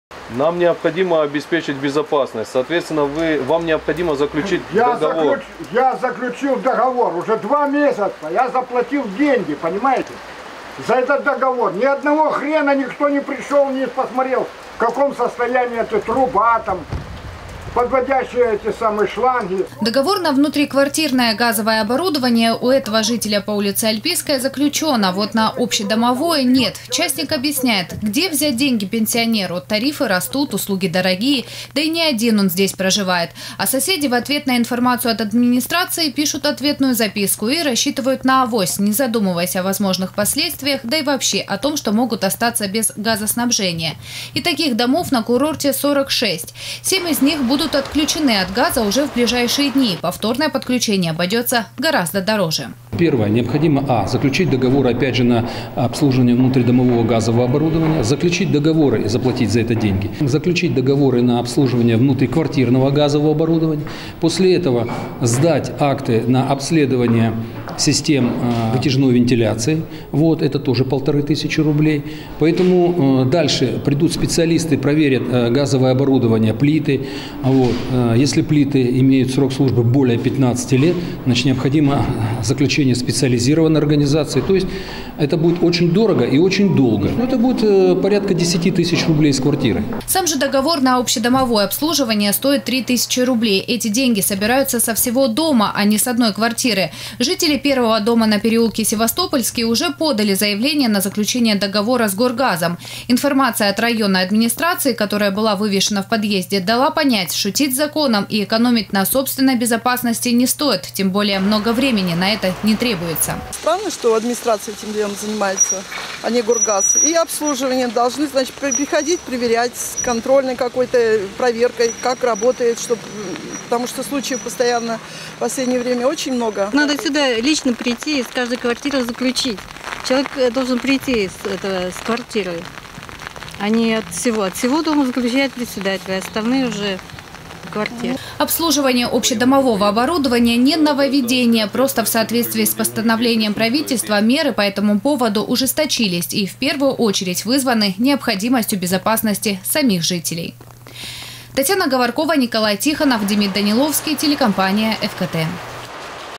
Без договора на газ сочинцы рискуют остаться без голубого топлива. Новости Эфкате Сочи
Соответствующее правило предоставления коммунальных услуг утверждено Правительством. Тем не менее, десятки домов на курорте не заключили договоры с эксплуатирующей организацией и являются повышенным источником опасности. О необходимости принятия мер жителей неоднократно информируют сотрудники районных администраций. Результаты есть.

Сочи. Новости.
Телеканал Эфкате.